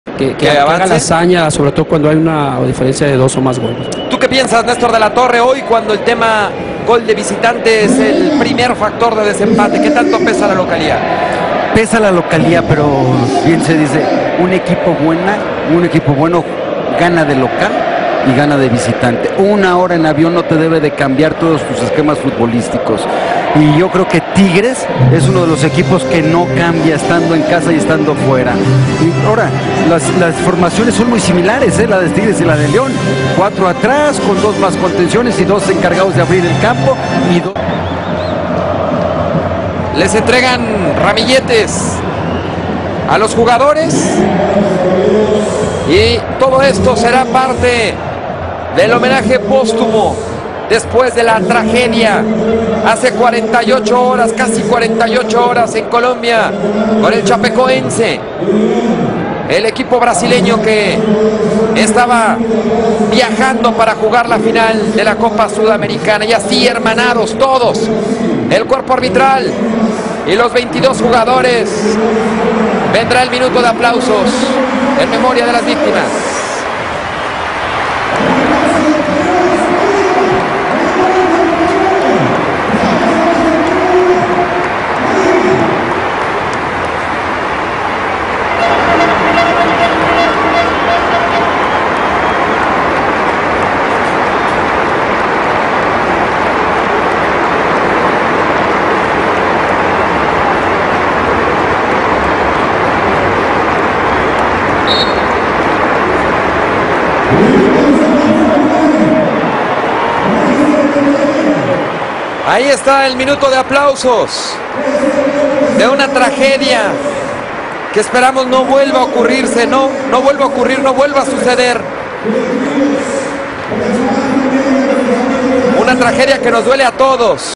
Así fue el homenaje del fútbol mexicano para el Chapecoense
En el estadio de León, antes del duelo ante Tigres, se honró a las víctimas del accidente aéreo que involucró al Chapecoense. Hubo pañuelos blancos, cánticos y los dos equipos hicieron juntos el minuto de silencio.